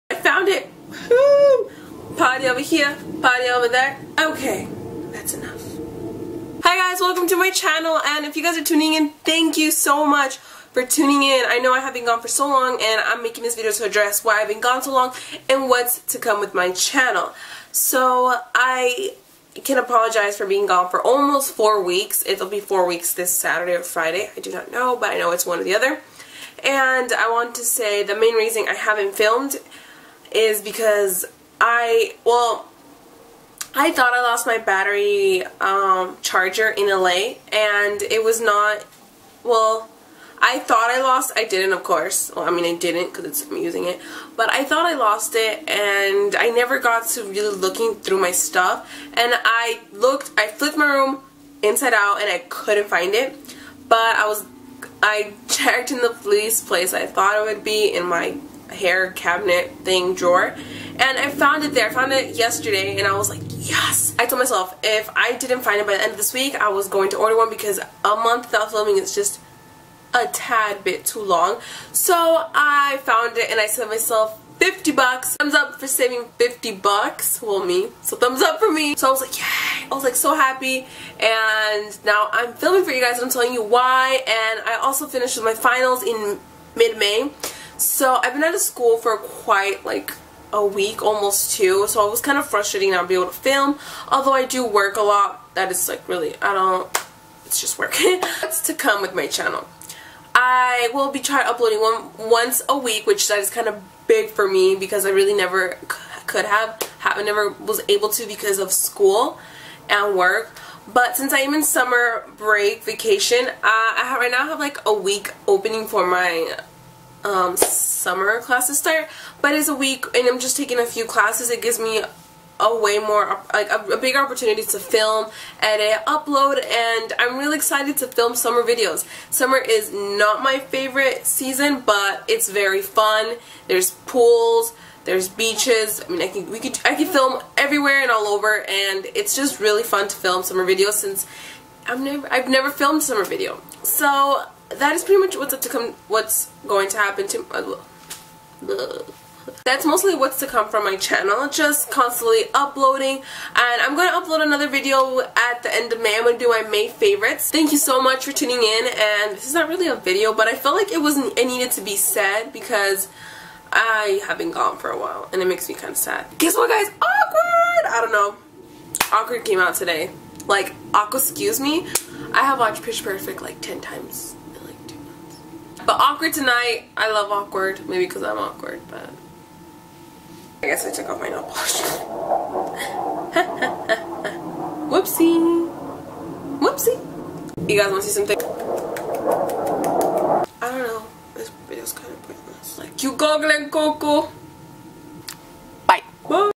Why I haven't uploaded?!?!
HI internet friends!
I want to apologize for being MIA for almost a month. Ahhhh! Almost a freaking MONTH! So, I wanted to post this video to let everyone know why I was unable to film a video.
I hope you guys enjoy the video, even though it's just a bunch of randomness.